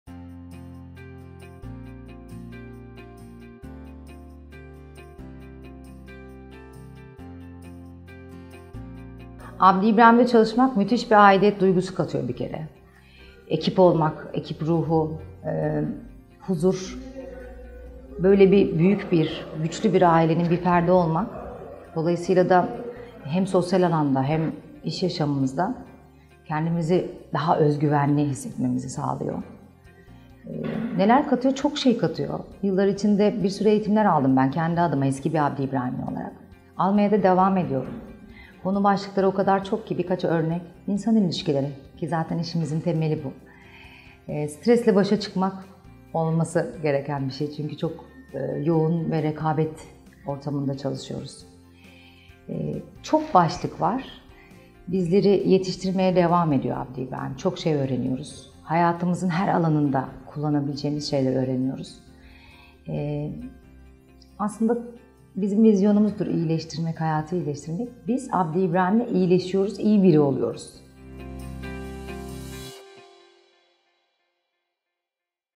Abdi İbrahim'de çalışmak size neler katıyor ve burada çalışırken neler öğreniyorsunuz?
Abdi İbrahim Tıbbi Tanıtım Temsilcisi Yeliz Kalkan Anlatıyor!

Abdi İbrahim çalışanları "Hayatı İyileştirenler" olarak topluma nasıl katkılar sağlıyorlar?